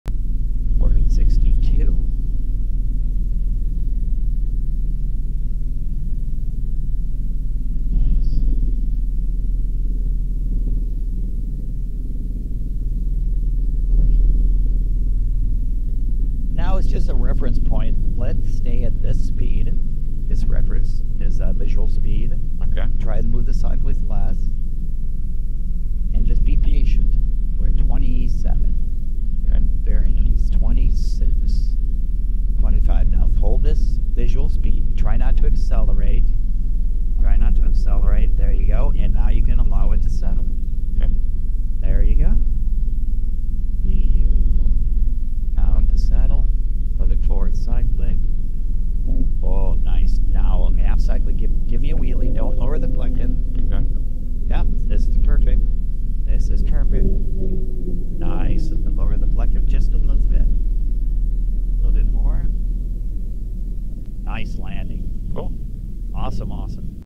#296 - EC130 Stuck pedal Landing (Loss of Control)
Knowledge is safety in the world of aviation.

Continuing Education